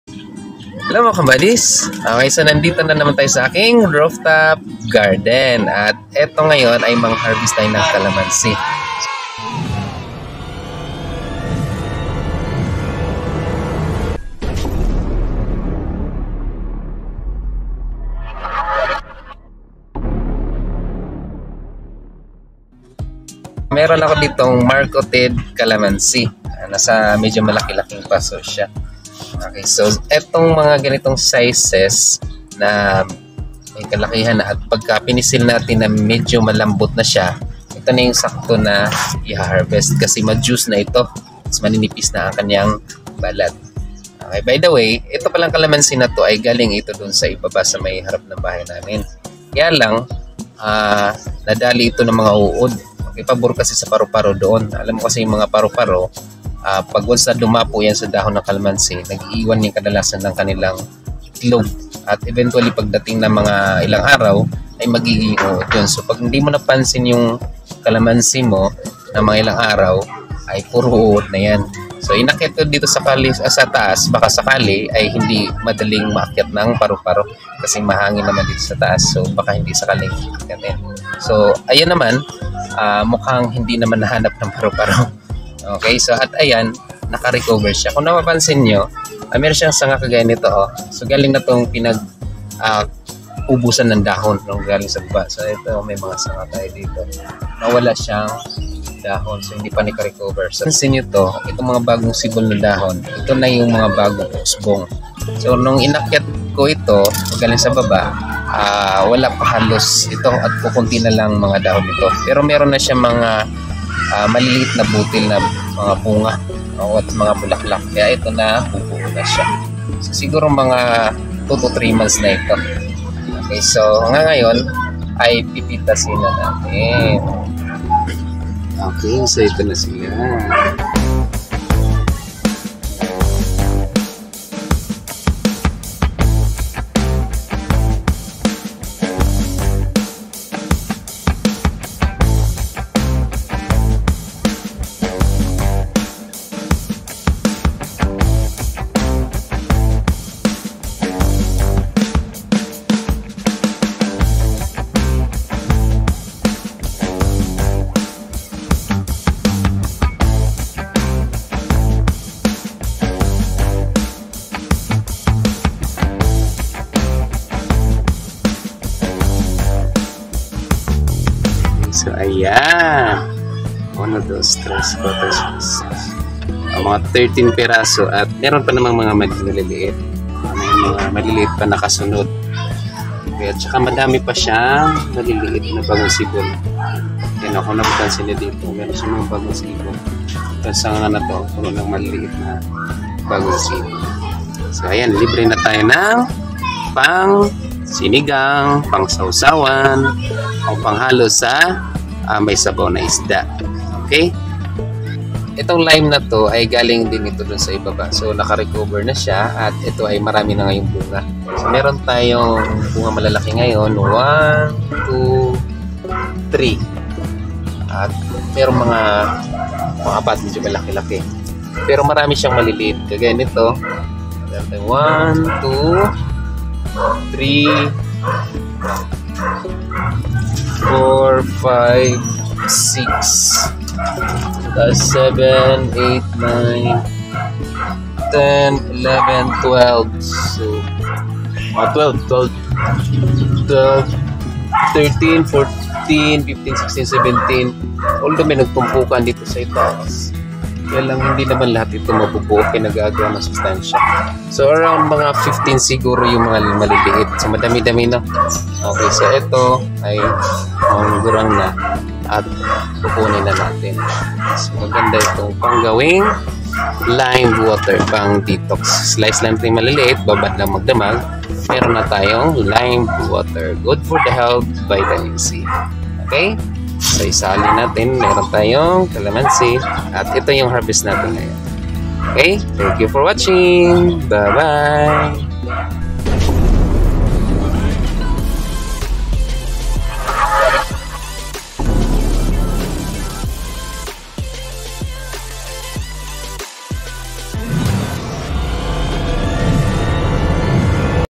HARVESTING KALAMANSI AND LIME AT ROOF TOP GARDEN
Join me on harvesting my kalamansi & lime at my roof top garden